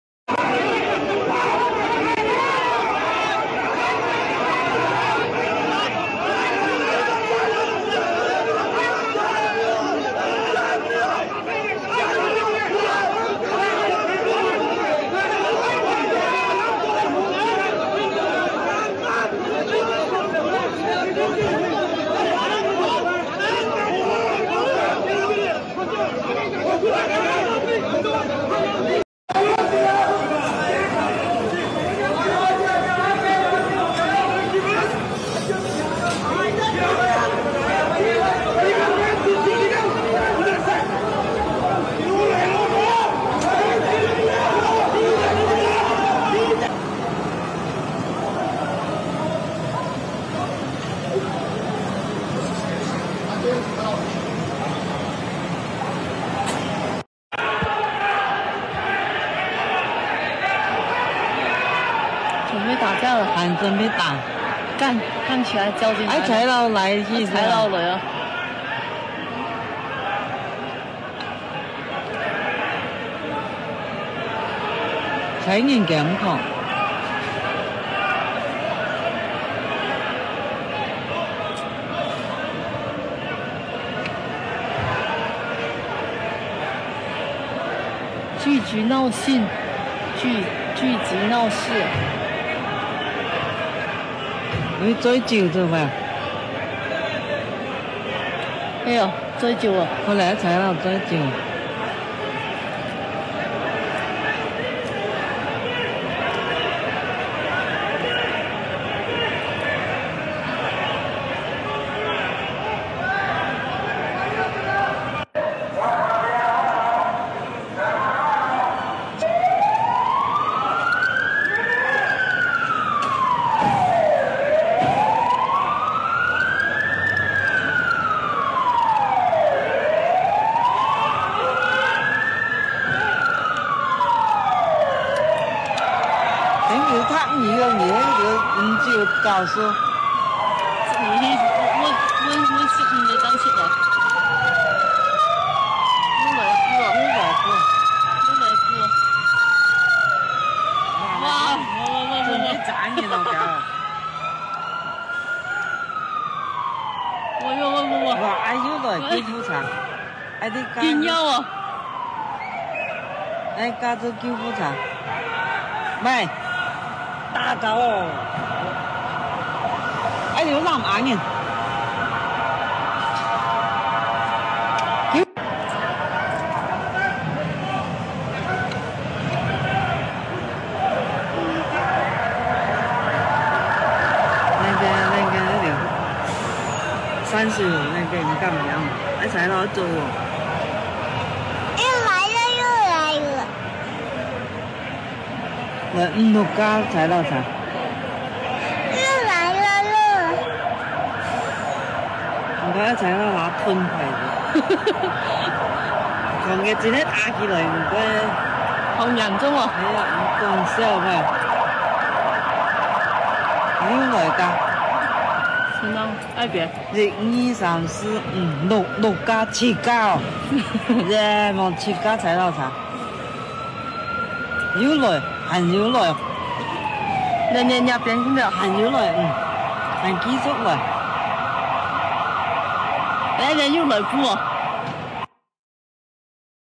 直擊 - 深水埗南亞群聚 圍警阻去路
估計是2023年3月27日深夜
南亞群聚，入鄉隨俗講DLLM
好像請一位南亞重要人物上車引發圍警

轉自: 反斗星